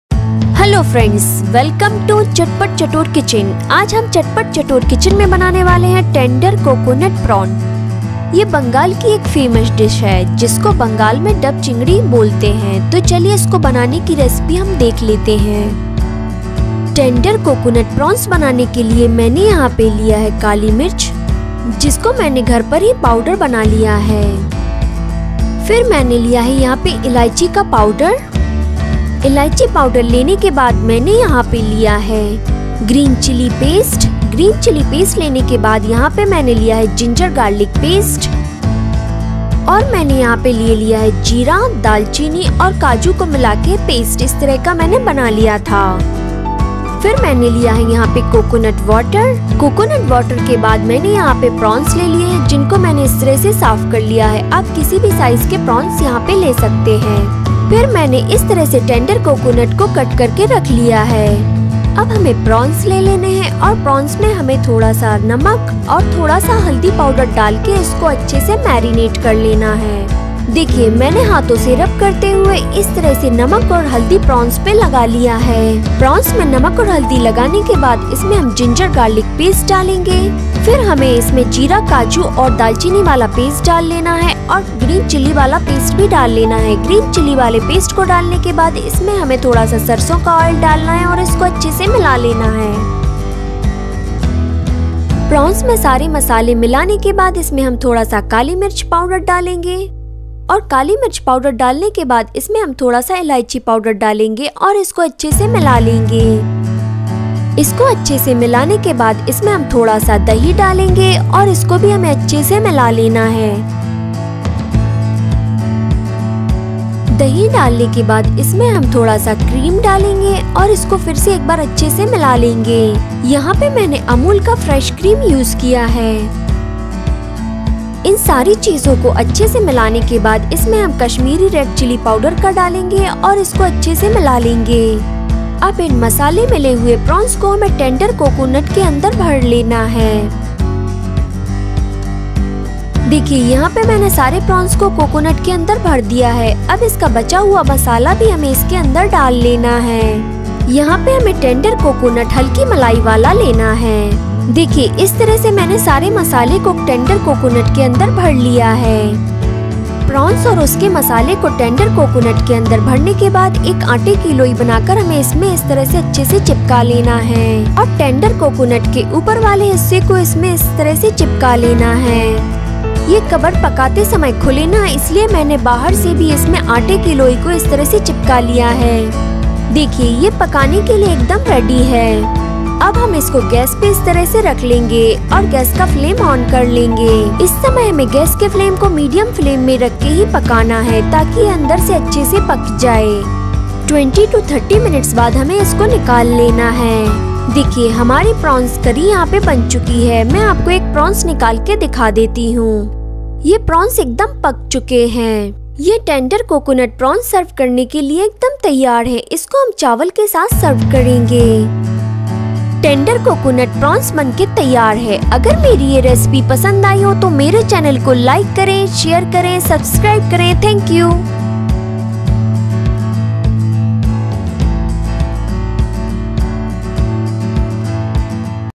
TENDER COCONUT PRAWN CURRY | SHRIMP CURRY| BY CHATPAT CHATTOR KITCHEN
INGREDIENTS AVAILABLE ONLINE

TURMERIC POWDER
RED CHILLI POWDER
BLACK PEPPER POWDER
MUSTARD OIL

INGREDIENTS FOR COOKING
 1 SPOON SALT
1/2-1 SPOON TURMERIC POWDER
1/4 SPOON RED CHILLI POWDER
1/4 SPOON BLACK PEPPER POWDER
1 SPOON CUMIN +CINNAMON +KAJU PASTE
1/4 SPOON CARDAMOM
2-3 SPOON MUSTARD OIL
1/2 SPOON GREEN CHILLI PASTE
3-4 PRAWNS
TENDER COCONUT
1-2 SPOON FRESH CREAM
1 SPOON CURD

IF ANYBODY HAVING ANY PROBLEM/ISSUES RELATED TO MY VIDEOS